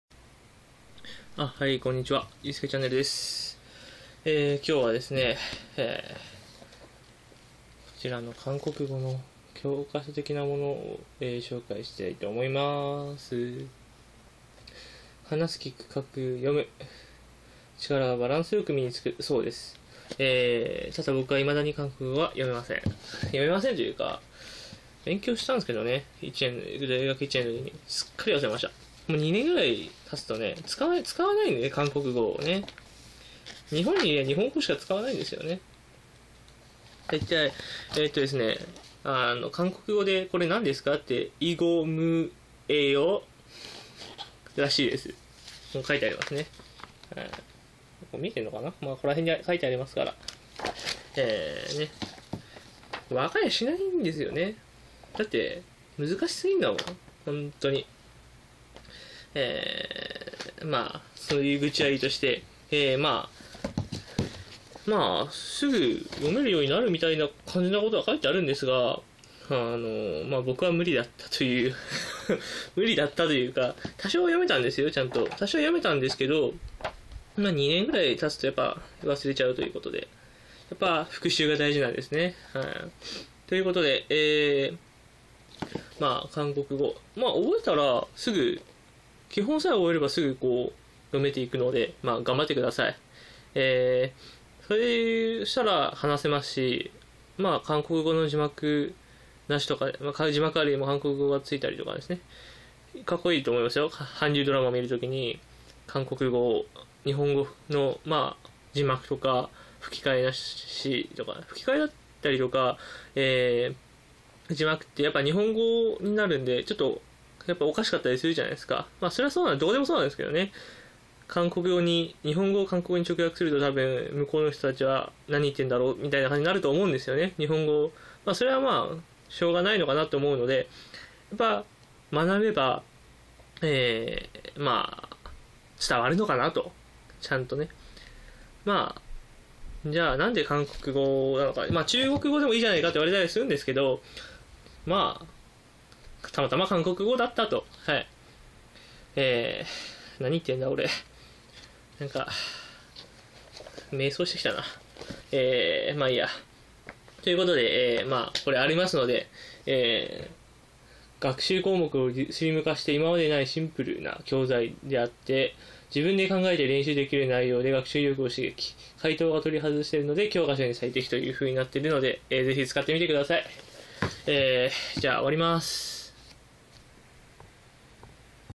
勉強しやすい！！ シンプル韓国語入門編 光化門韓国語スタジオ著
韓国語が学べる参考書です。
最小の学習項目で最大の学習効果を得られるようです。
一回勉強したら、復習は忘れずに！